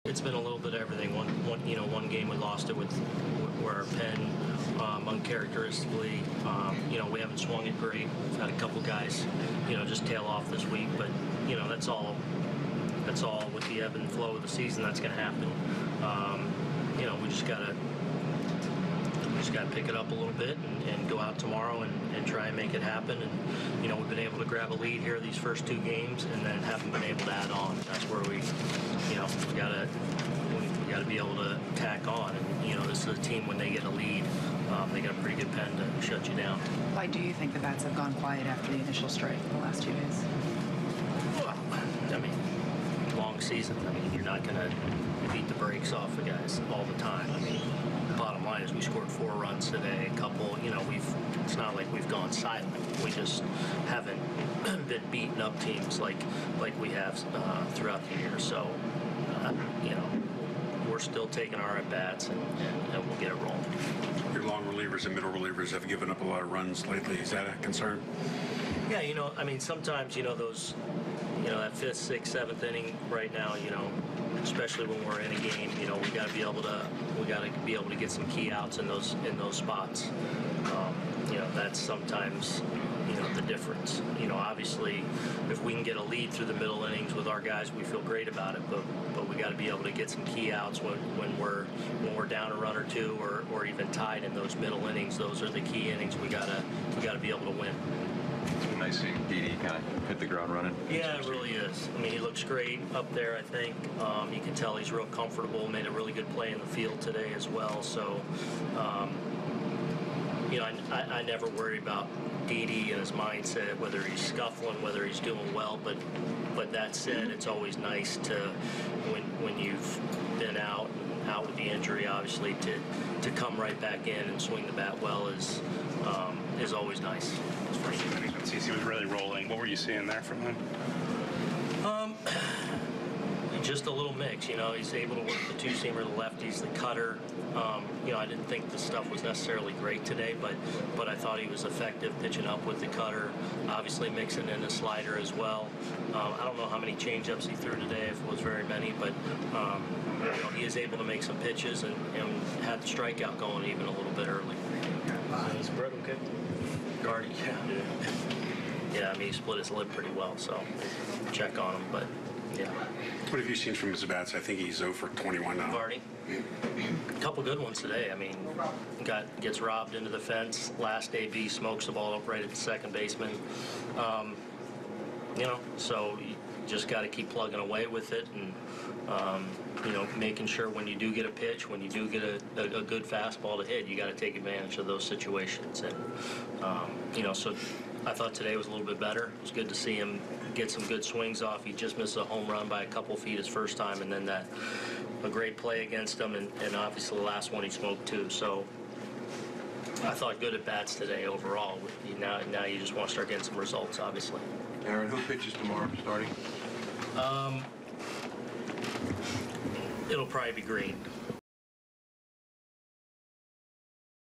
Aaron Boone reacts to Yankees' 8-4 loss vs. Cleveland
Yankees drop their second straight series as they fall to the Indians 8-4 Saturday in Cleveland.